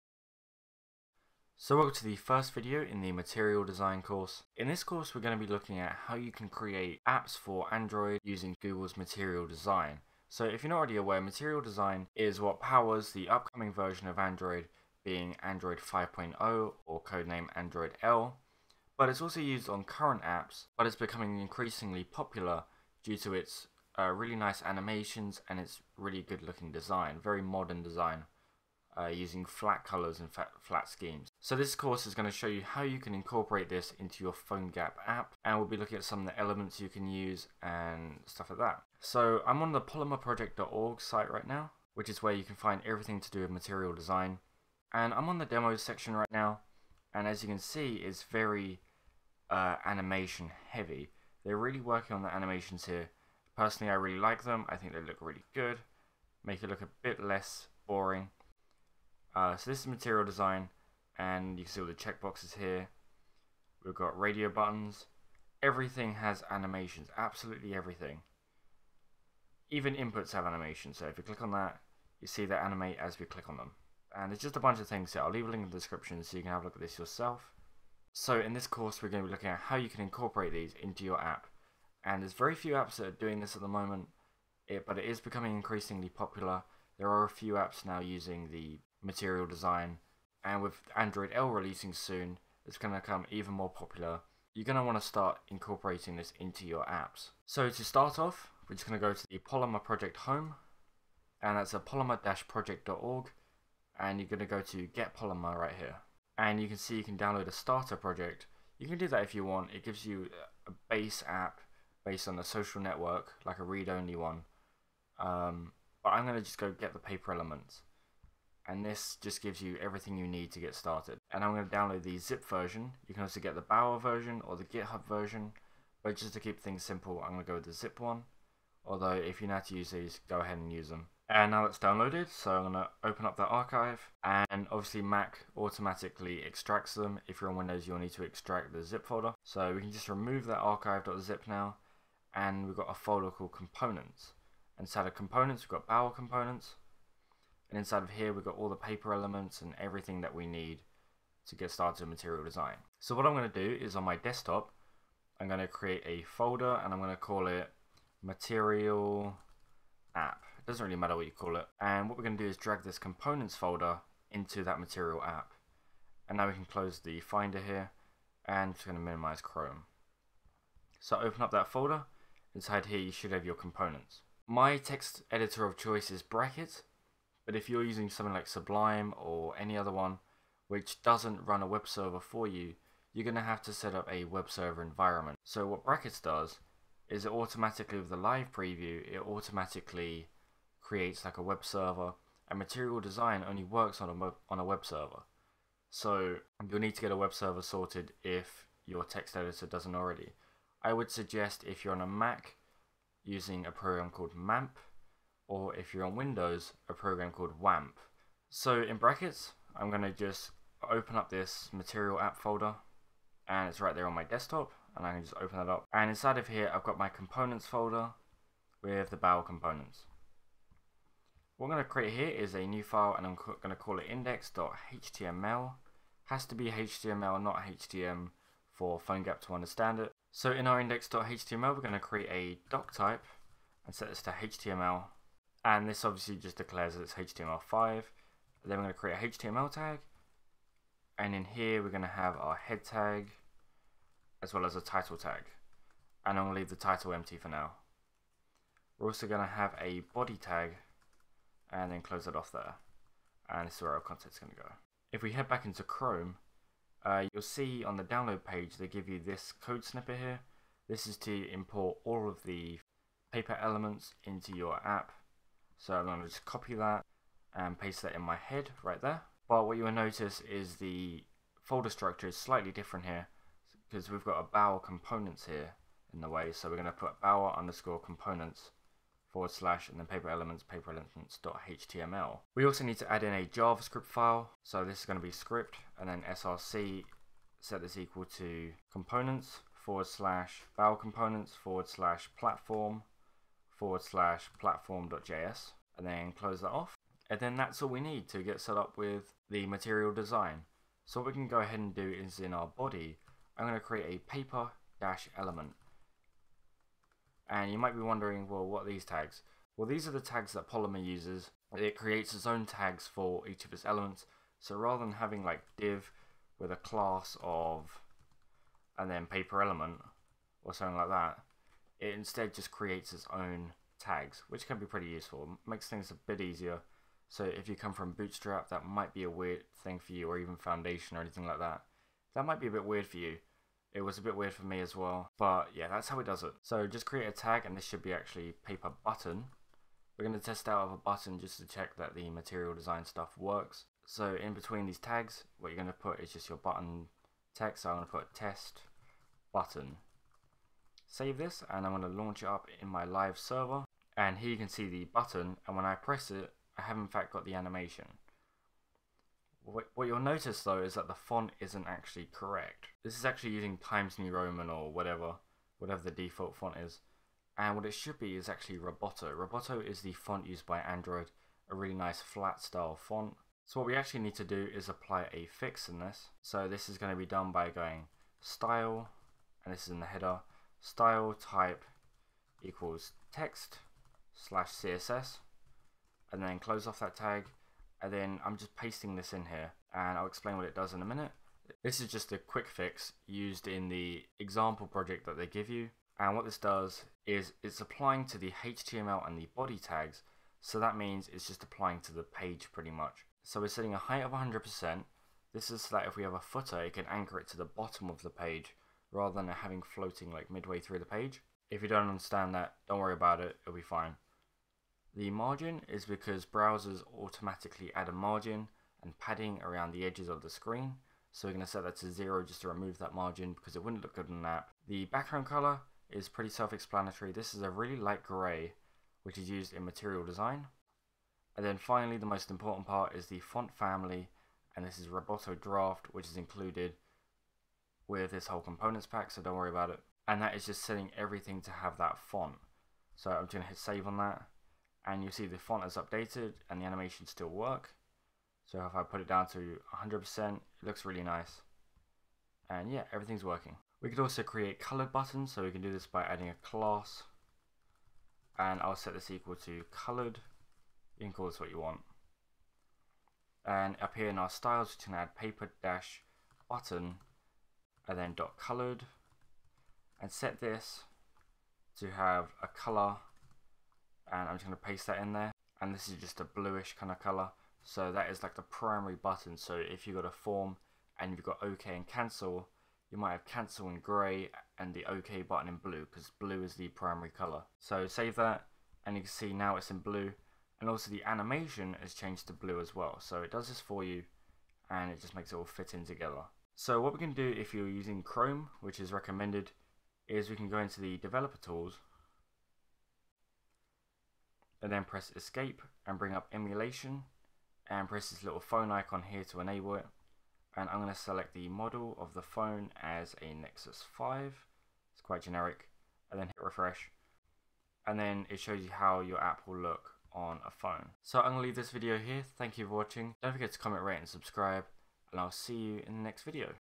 PhoneGap + Android Material Design - #1 - Setup [Deprecated]
Unfortunately, updates to Polymer mean that this tutorial may no longer be relevant.

This is the first video in a new course, where we'll build a fully functioning app with Android's new material design. In this video, we'll be setting up Polymer, and I'll show you how to create buttons to test that everything is working.

If you'd like to test your project as an app on a device, the recommending way is to compress your app's folder into a zip file and upload it to the PhoneGap Build service. This will be in a future tutorial.

Material design is new for Android 5.0, codenamed 'Android L', although it is becoming increasingly popular among apps for previous versions of Android.